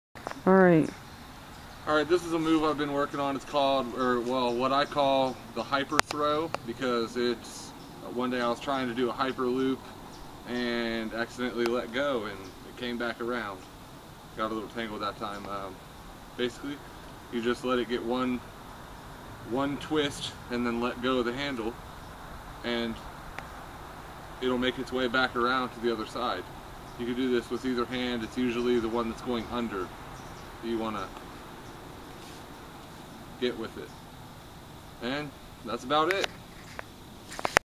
Poi - Hyper Throw
This is a move I came up with one day completely by mistake but I liked it. What started out an error, after some practice became a trick. I haven't really seen anyone else do this and if so I'm not sure if there's a name for it but I've been referring to it as a Hyper Throw.

This can be accomplished by spinning the poi in split time same direction and crossing the tethers as if to do a Hyperloop. As soon as they make one twist let go of the handle of the poi that will be traveling under the other one. If done correctly the poi should stay tangled long enough to do one full rotation and then be easily pulled loose by the other hand.

Other Variations I've messed with:
Behind the Back
Behind the Neck
Under the Legs (Be careful guys.. lol.. I speak from experience)

A bit more difficult but possible:
By slowing down the rotation of the poi that you're still holding as it's making it's upswing, the second one can be released in an upwards throw. This almost deserves a video all to itself, but I haven't completely mastered that one yet.